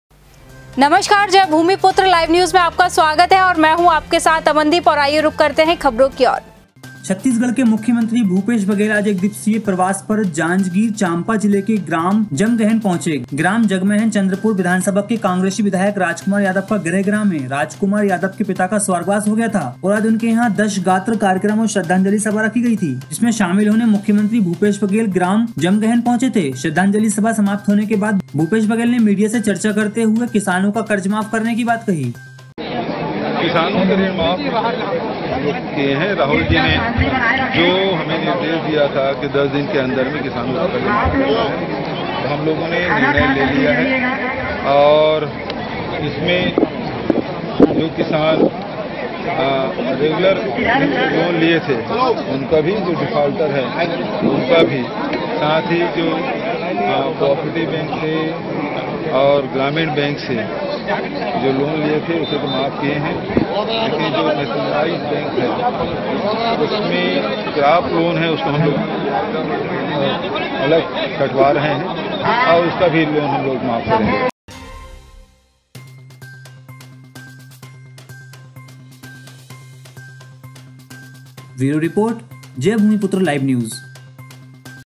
छत्तीसगढ़ के मुख्यमंत्री भूपेश बघेल ने की मीडिया से मुलाकात
छत्तीसगढ़ के मुख्यमंत्री भूपेश बघेल आज एक दिवसीय प्रवास पर जांजगीर चांपा जिले के ग्राम जमगहन पहुँचे, ग्राम जमगहन चंद्रपुर विधानसभा के कांग्रेसी विधायक रामकुमार यादव का गृह ग्राम है, रामकुमार यादव के पिता का स्वर्गवास हो गया था और आज उनके यहाँ दशगात्र कार्यक्रम और श्र्द्धांजलि सभा रखा गया था ,जिसमे शामिल होने मुख्यमंत्री भूपेश बघेल ग्राम जमगहन पहुँचे थे, श्र्द्धांजलि सभा समाप्त होने के बाद भूपेश बघेल ने मीडिया से चर्चा करते कहा कि किसानो का कर्ज माफ़ करने की बात कही